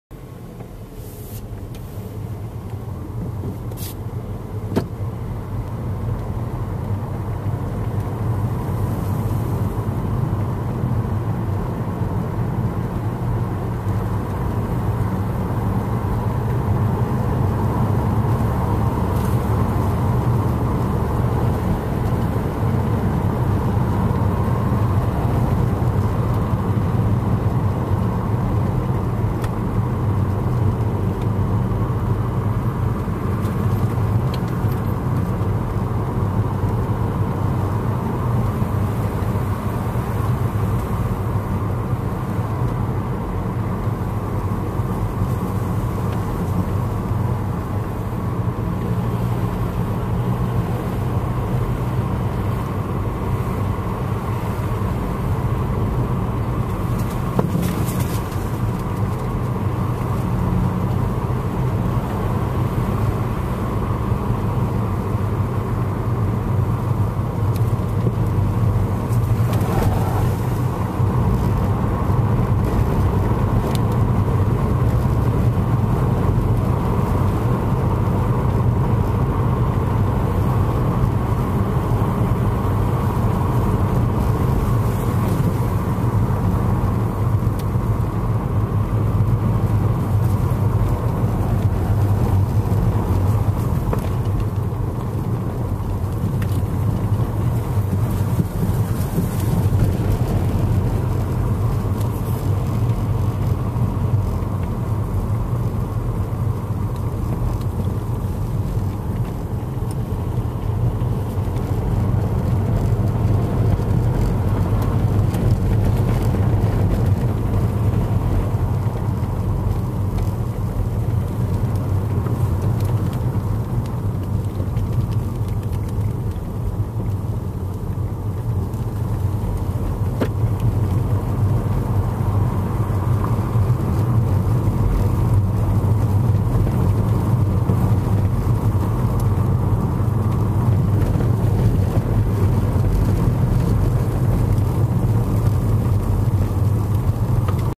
Autumn Drive West On South Road Tummel Highland Perthshire Scotland
Tour Scotland video of an Autumn drive West on the South road by Loch Tummel, Highland Perthshire.